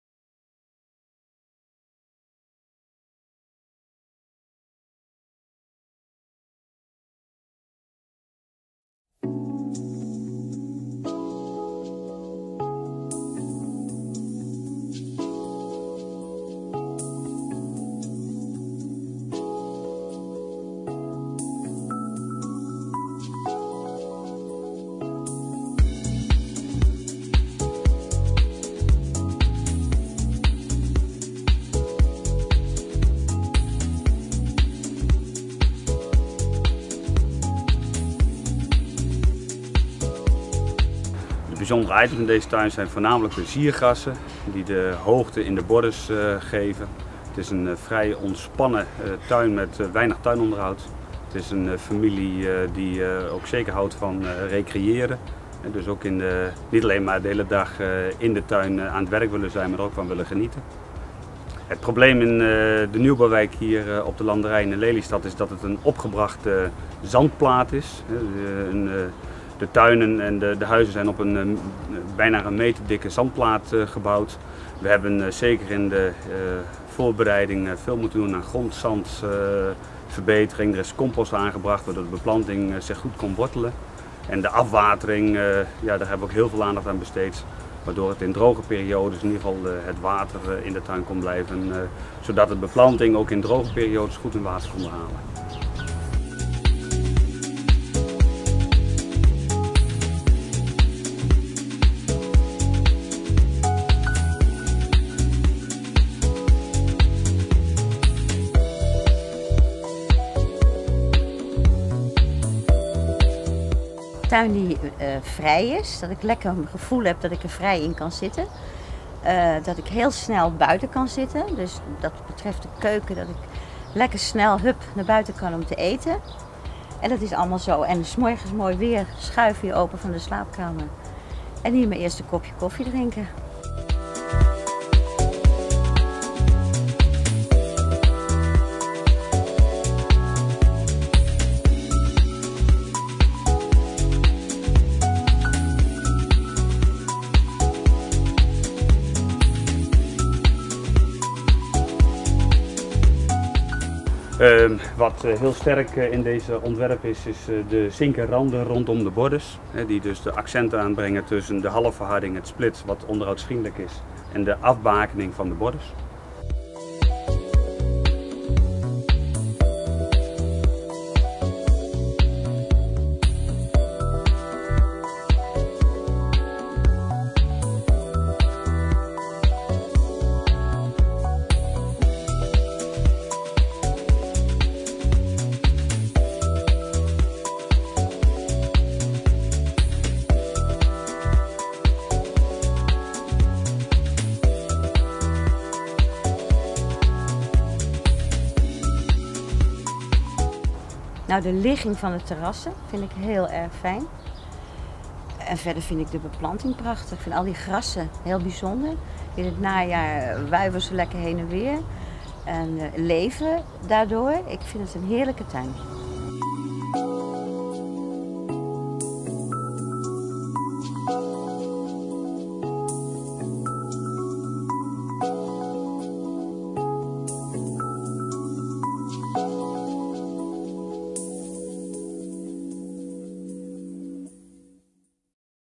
Moderne leeftuin in Lelystad.mp4
Genomineerde tuin

Hoveniersbedrijf All in Tuinen uit Kraggenburg nomineert zich met moderne leeftuin in Lelystad